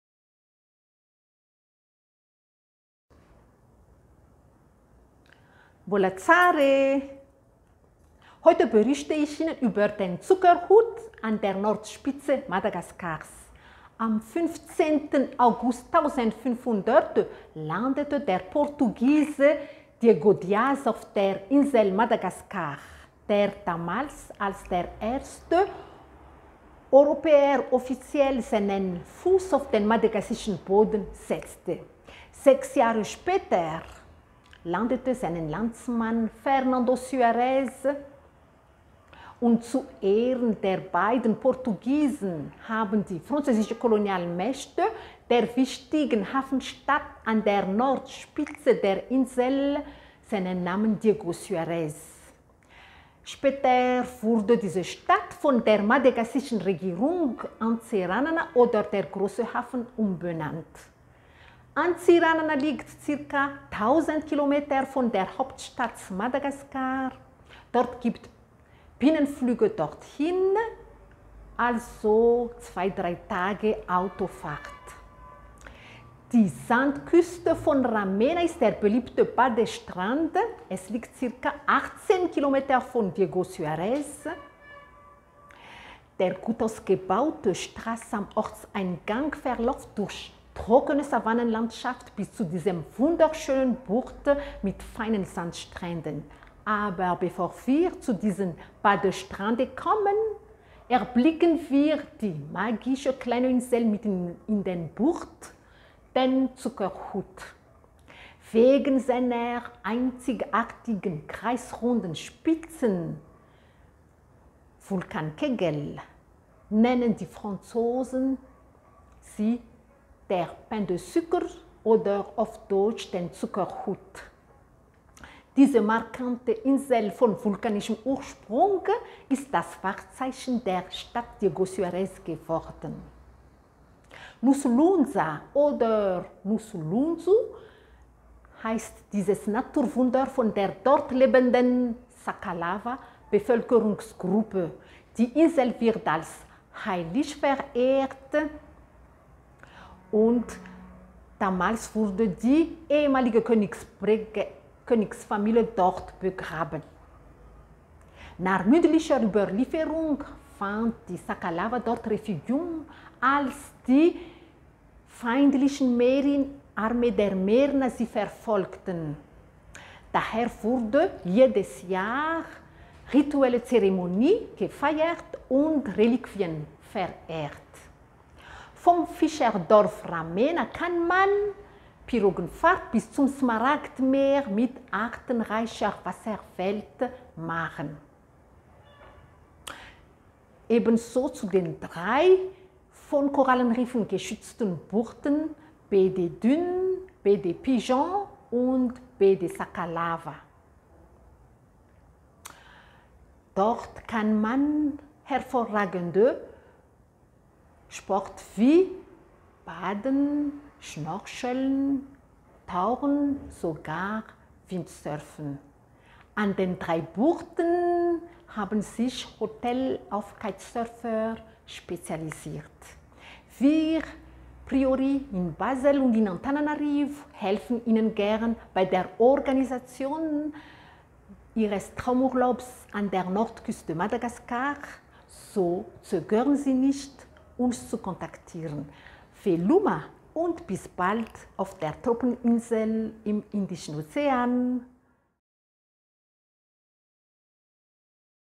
Zuckerhut in Diego Suarez Madagaskar
Mbola Tsara e

Heute berichte ich über den Zuckerhut an der Nordspitze von Madagaskar.

Am 10. August 1500 landete der Portugiese Diego Diaz auf die Insel Madagaskar, der damit als als erster Europäer offiziell seinen Fuss auf madagassischen Boden setzte. Sechs Jahre später kam auch sein Landsmann, der portugiesische Seefahrer Fernando Suarez. Zu Ehren dieser beiden Portugiesen gaben die französischen Kolonialmächte der wichtigen Hafenstadt an der Nordspitze der Insel den Namen Diego Suarez. Später wurde diese Stadt von der madagassischen Regierung in Antsiranana oder der „grosse Hafen“ umbenannt.

Antsiranana liegt rund 1000 km von der Hauptstadt Madagaskars entfernt. Es gibt Binnenflüge dorthin – oder zwei, drei Tage Autofahrt.

Die Sandküste von Ramena ist der beliebteste Badestrand. Er liegt rund 18 km östlich von Diego Suarez. Die gut ausgebaute Strasse am Ortseingang von Diego Suarez verläuft durch eine trockene Savannenlandschaft bis zu dieser wunderschönen grossen Bucht mit weissen Sandstränden. Aber bevor wir diesen Badestrand erreichen, erblicken wir eine magische, kleine Insel mitten in der Bucht. Den Zuckerhut.

Wegen seiner einzigartigen kreisrunden und spitzen Form haben die Franzosen diesen Vulkankegel “Pain de Sucre“ genannt, auf deutsch „Zuckerhut“. Diese markante Insel mit vulkanischem Ursprung ist das Wahrzeichen der Stadt Diego Suarez geworden.

„Nosy Lonja“ oder „Nosy Lonjo“ heisst dieses Naturwunder bei der dort lebenden Sakalava-Bevölkerungsgruppe. Die Insel wird als heilig verehrt, da die damalige Königsfamilie aus Diego Suarez dort begraben wurde.

Nach mündlicher Überlieferung fanden die Sakalava auch ein Refugium auf dieser Insel, als die feindlichen Armeen der Merina sie verfolgten. Daher werden dort jedes Jahr rituelle Zeremonien gefeiert und Reliquien verehrt.

Vom nahegelegenen Fischerdorf in Ramena kann man eine Bootsfahrt bis zum türkisblauen Smaragdmeer mit einer artenreichen Wasserwelt machen. Ebenso zu den drei von Korallenriffen geschützten Buchten „Baie des Dunes“, „Baie des Pigeons“ und „Baie des Sakalava“. Dort gibt es hervorragende Möglichkeiten zum Baden, Schnorcheln, Tauchen und besonders Windsurfen. An diesen drei Buchten haben sich Hotels auf Kitesurfer spezialisiert.

Wir von PRIORI in Basel und in Antananarivo helfen Ihnen gern bei der Organisation Ihres Traumurlaubs an der Nordküste Madagaskars. Zögern Sie nicht, uns zu kontaktieren.

Veloma und bis bald auf der Trauminsel im Indischen Ozean.